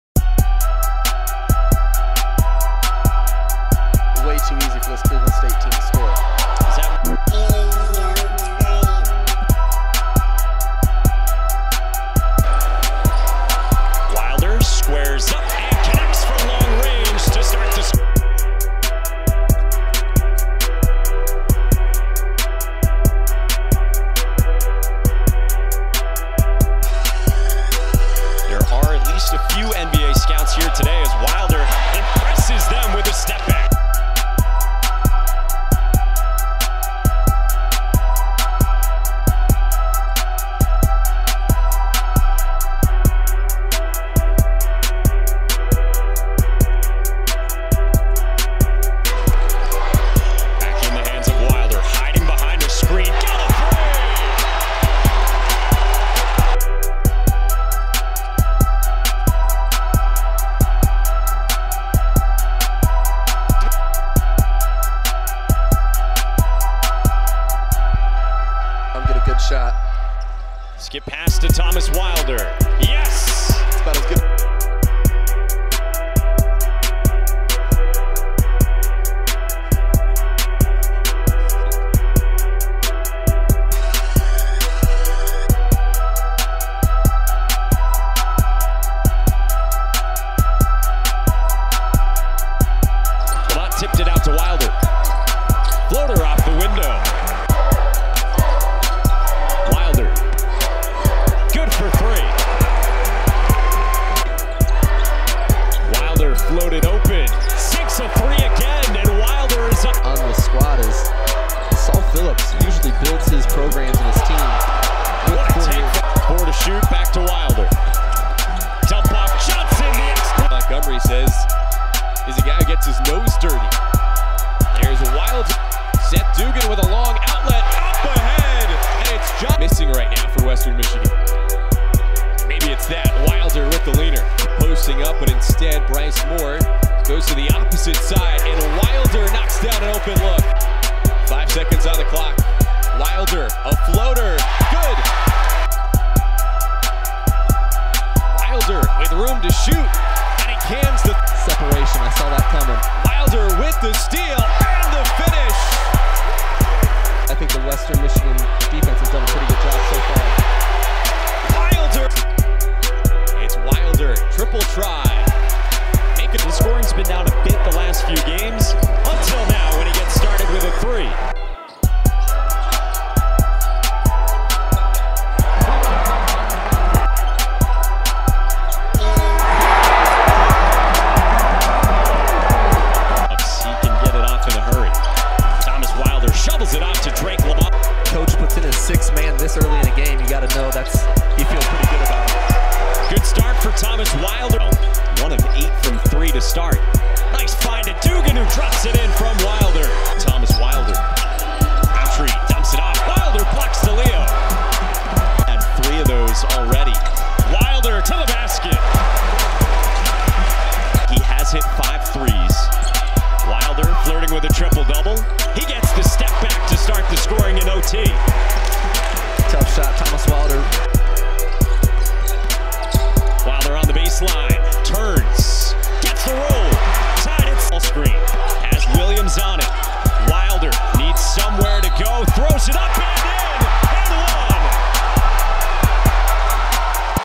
Thomas Wilder 2017-2018 Highlight Tape
Senior Guard Thomas Wilder's Highlights from his final season at Western Michigan University